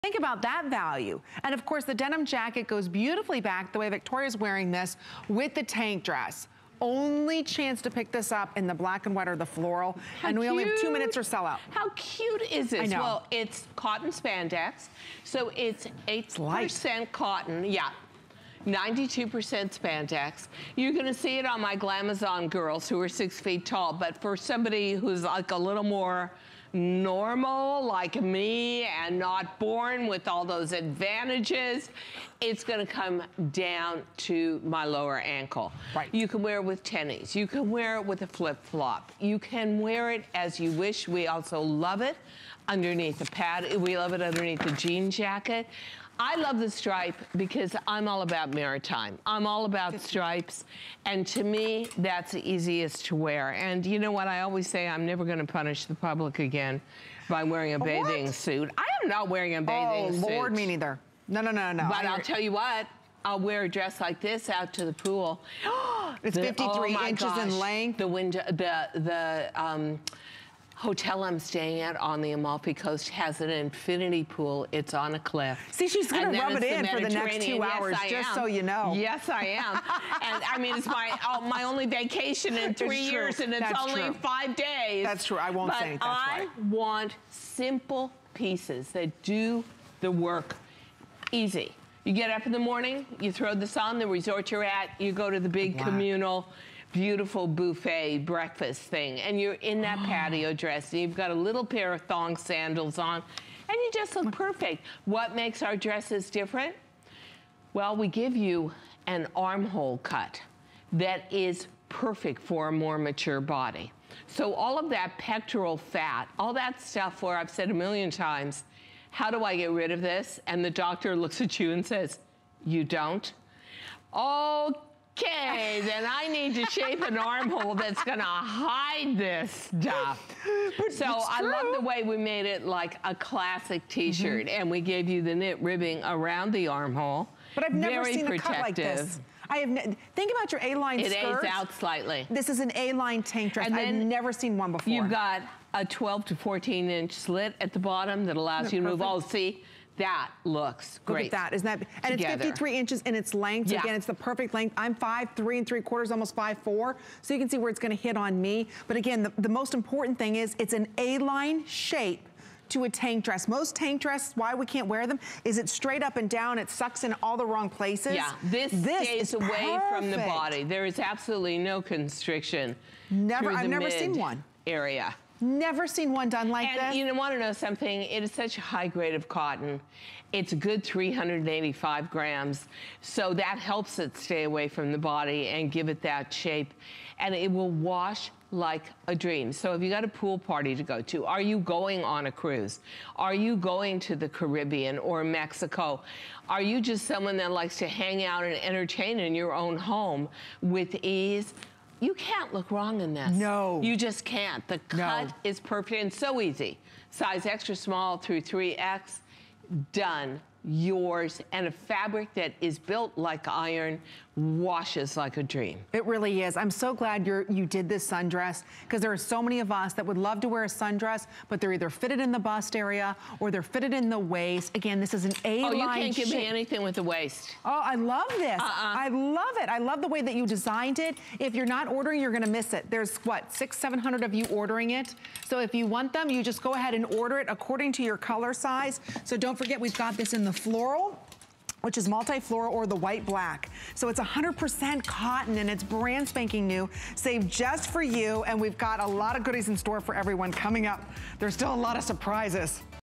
DG2 by Diane Gilman Printed Tank Maxi Dress
Whether vacation bound or just heading into the weekend, you'll love having this maxi dress on hand. Its easy, pullover style lets you get ready in a...